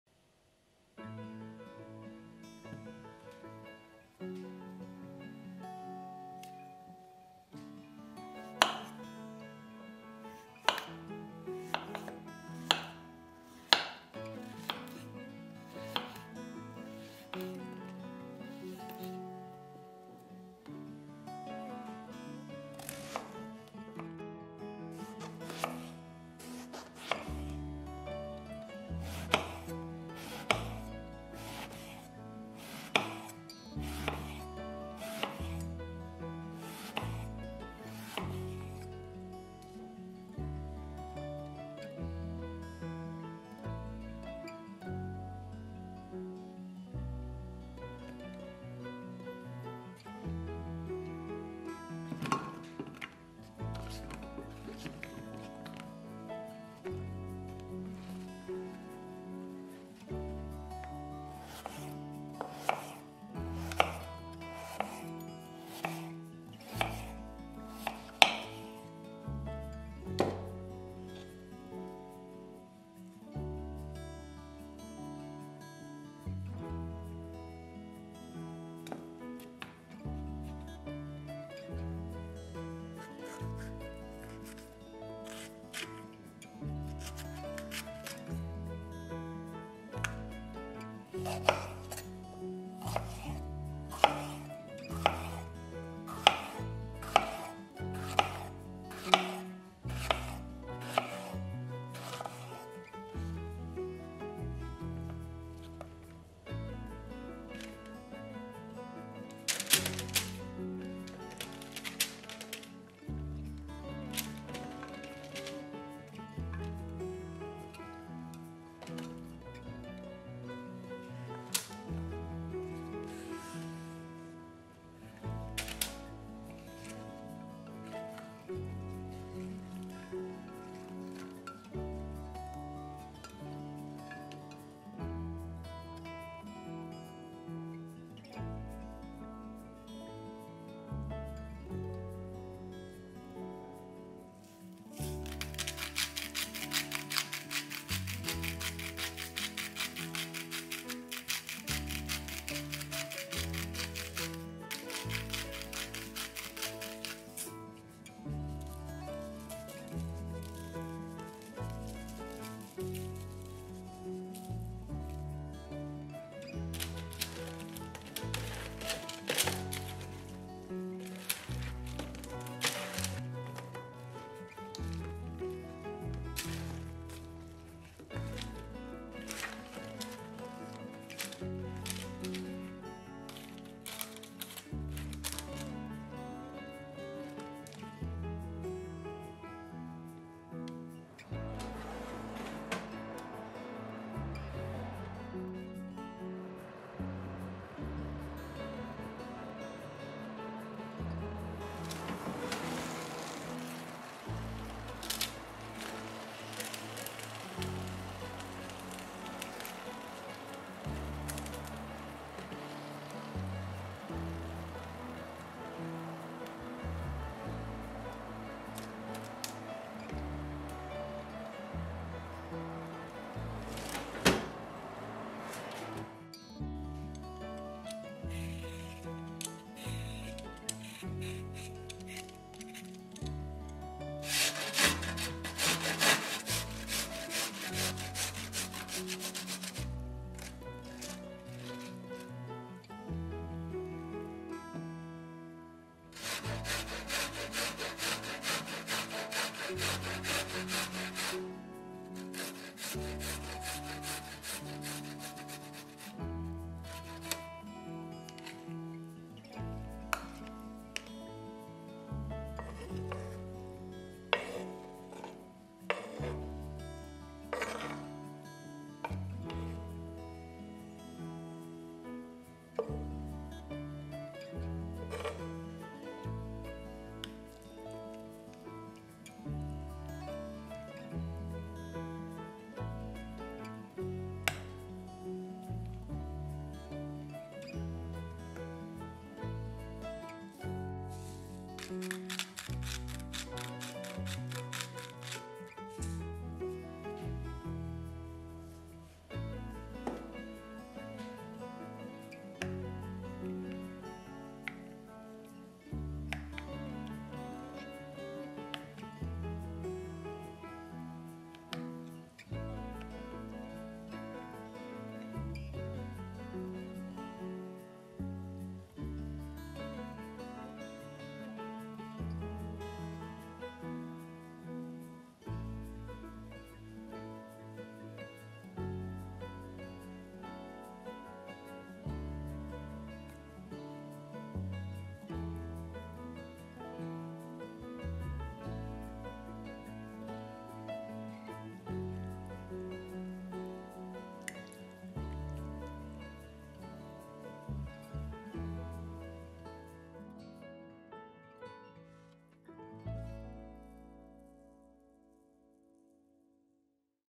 If you have zucchini, you need to make this recipe! Quick and delicious dinner!
grilled zucchini and eggplant
Thinly slice the zucchini and eggplant
2 potatoes cut into half rings
Cut the pepper in half and remove the white ribs
Cut the pepper halves into  strips
Add the sliced vegetables to a baking pan or sheet and cover with 1/2 cup olive oil.
Salt
I used my hands to make sure all the pieces are coated with the olive oil.
Bake 350°F 180°C for  25 mins or until tender
stir after 10 minutes
peel and grate cucumber
1 garlic clove grated
plain yogurt 5 tbsp
dill
salt
pepper mix

About Simple Food:
Here you can find many different recipes. Healthy recipes, desserts, dinner, lunch and breakfast recipes.